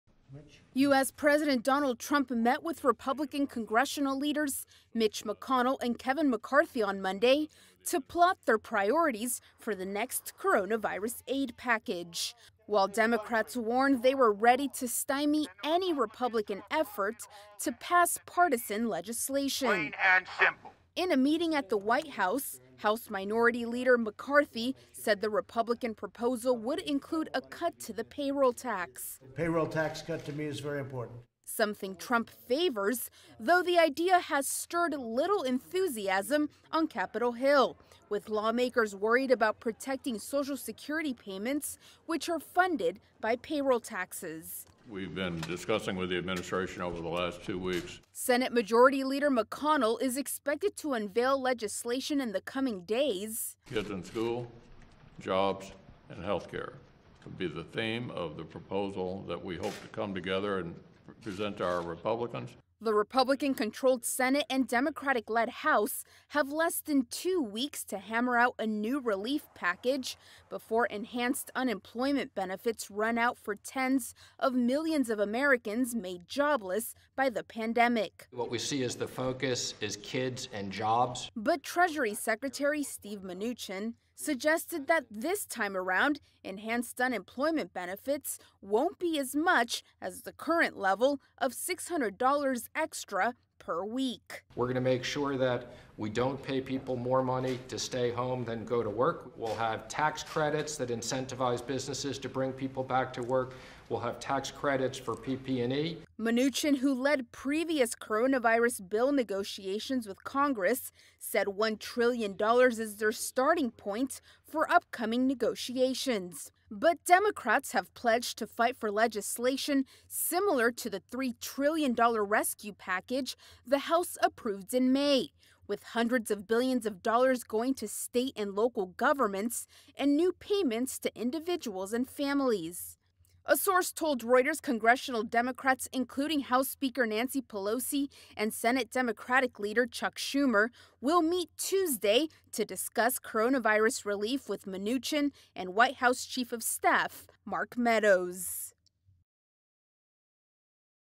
Republicans to push for $1 trillion aid package
U.S. President Donald Trump met with Republican congressional leaders Mitch McConnell and Kevin McCarthy on Monday to plot their priorities for the next coronavirus aid package - while Democrats warned they were ready to stymie any Republican effort to pass partisan legislation. In a meeting at the White House, House Minority Leader McCarthy said the Republican proposal would include a cut to the payroll tax. Something Trump favors, though the idea has stirred little enthusiasm on Capitol Hill, with lawmakers worried about protecting Social Security payments, which are funded by payroll taxes.